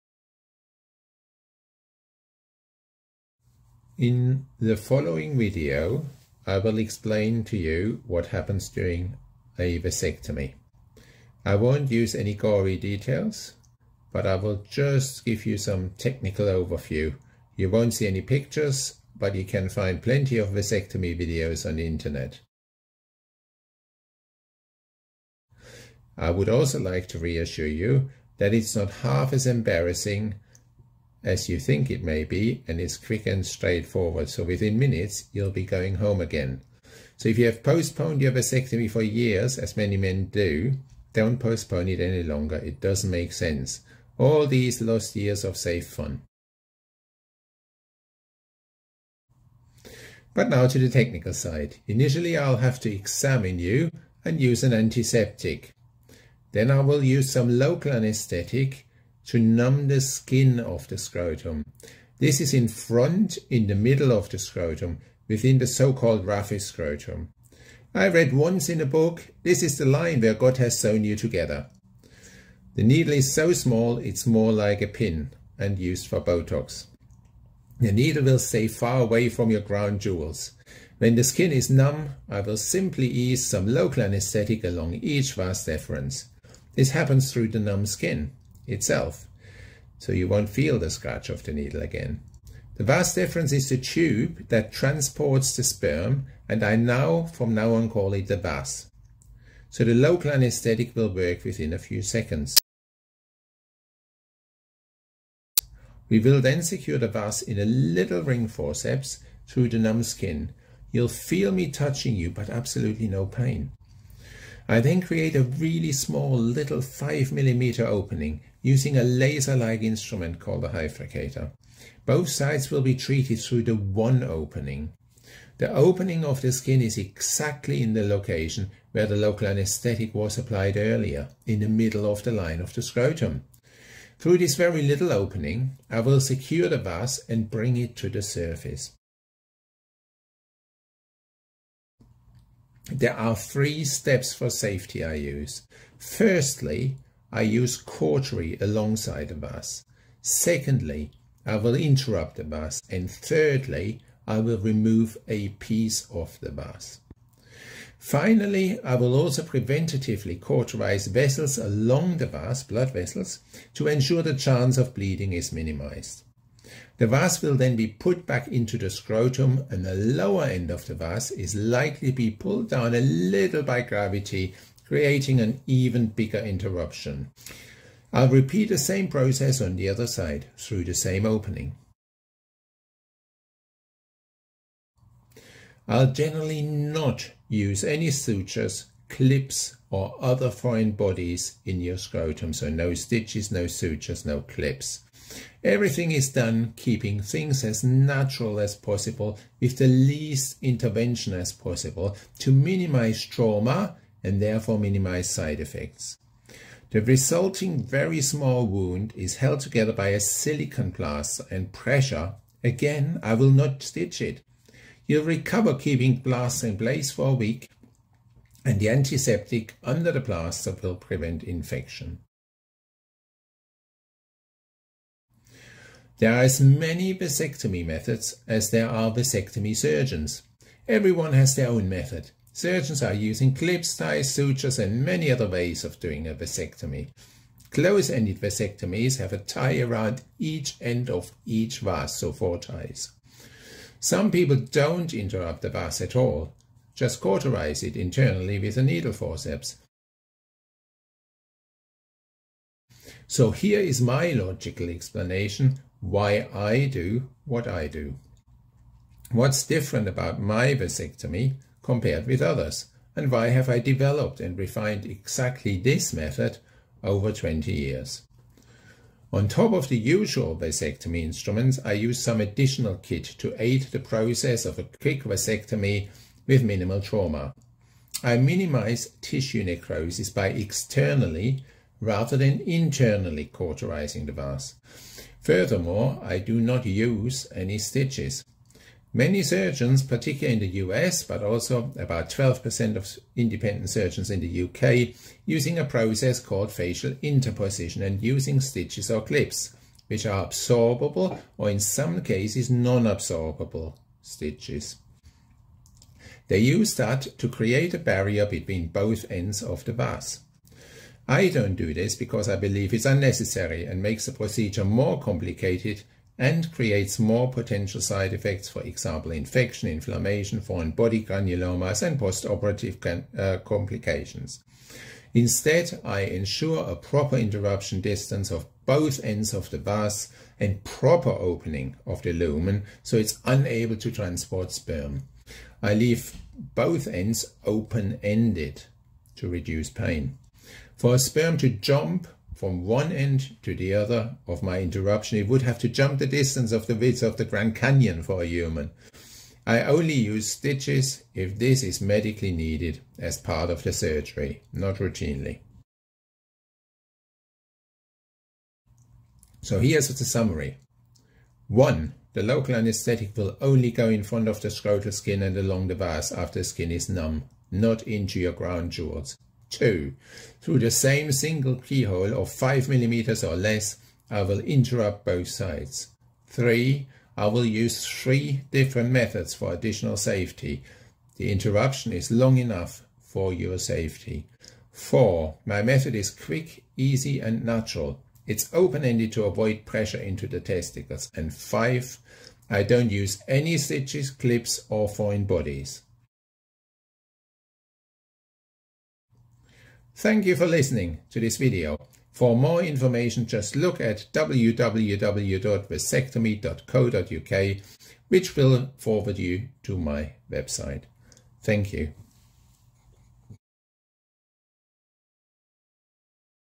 No scalpel vasectomy technique explained - no gory details
Dr Kittel explains his No-scalpel vasectomy method. He explains thoroughly all the steps of the vasectomy surgery and why he has developed this method over 20 years. He explains how the local anaesthetic is given, how he ensures an open-ended no-scalpel vasectomy method and how he minimises trauma. He gives logical reasons for each step during his no scalpel vasectomy. He explains why he avoids stitching and foreign bodies and how he ensures a safe and effective vasectomy.